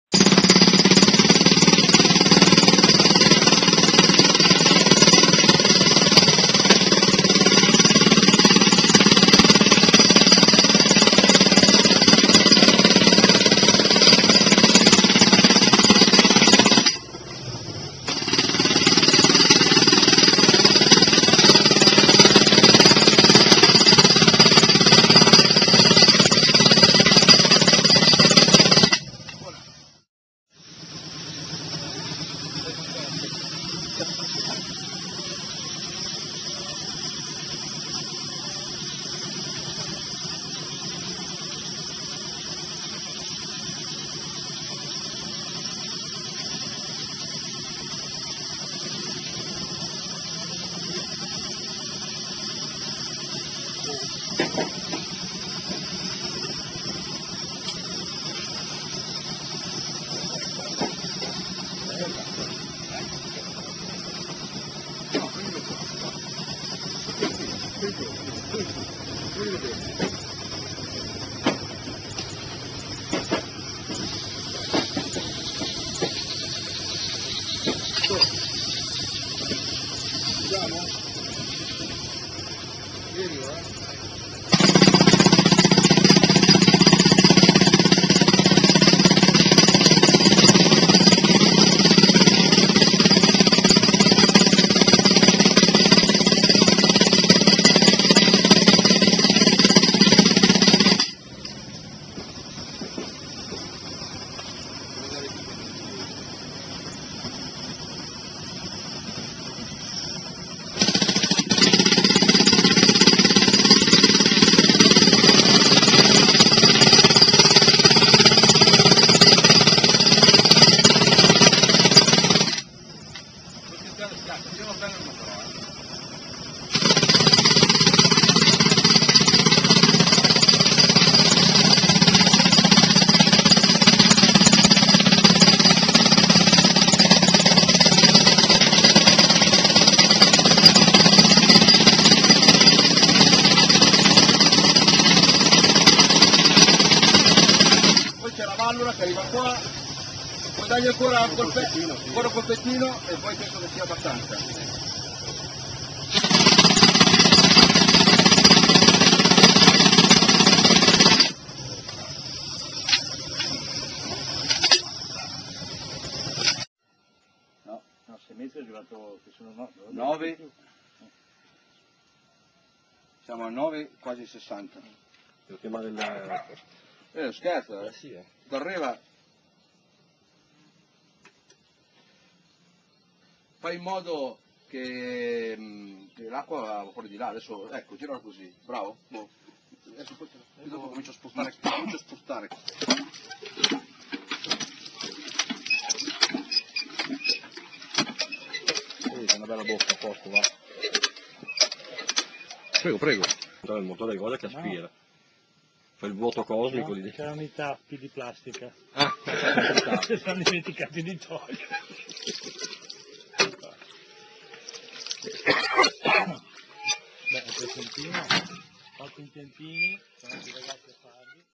pozzo per acqua
come si pianta un pozzo profondo 10 metri in tubo di ferro in un'ora con un costo conveniente.